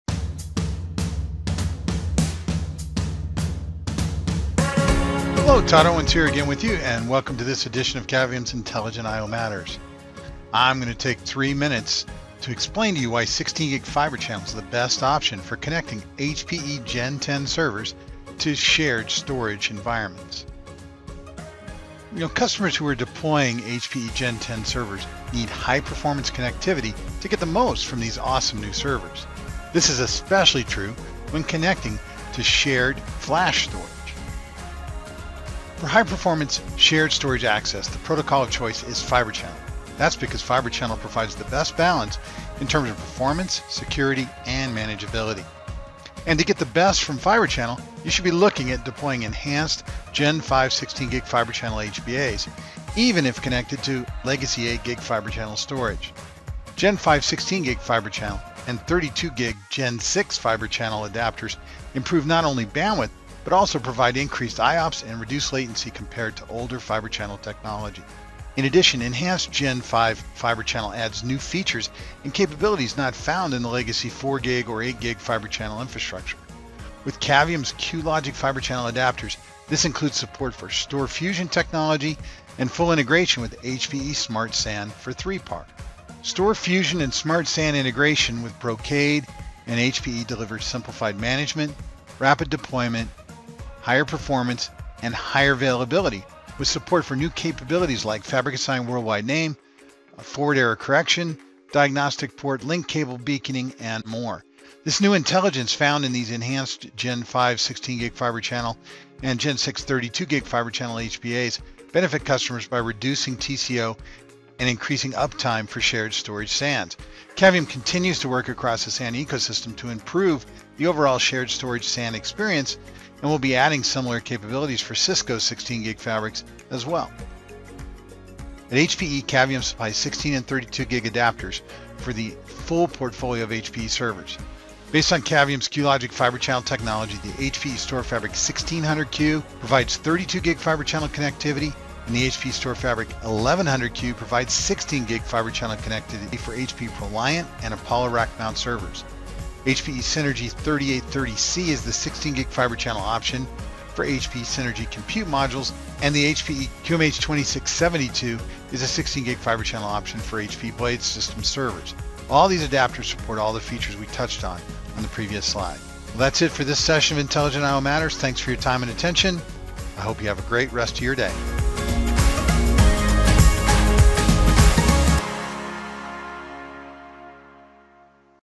Why 16Gb Fibre Channel for HPE Gen10 Servers
HPE's Gen10 servers provide a powerful compute platform for today’s modern data center. Watch this video to learn why Enhanced Gen5 16Gb Fibre Channel is the ideal option for connecting these servers to share storage SAN environments.